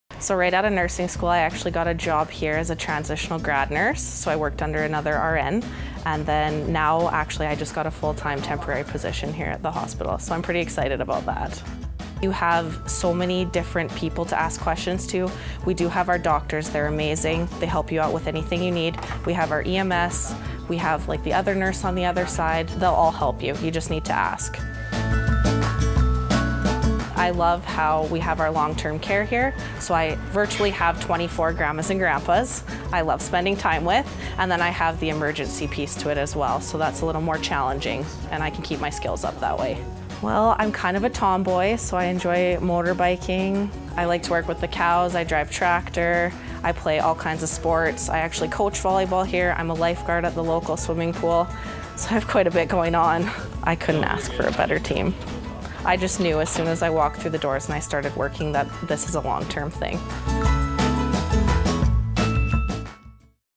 Bailey Balog Loves Being a Registered Nurse in Milk River
A self described tomboy, Bailey Balog loves the rural lifestyle of the Milk River area.  She lives on a farm with her husband.  So, don't be surprised to see her behind the wheel of a tractor or combine.  She's very active in sports, playing baseball, coaching volleyball and working at the local pool as a lifeguard.
Bailey practises at the Milk River Health Centre.  She enjoys being part of a healthcare team that puts patients' needs first.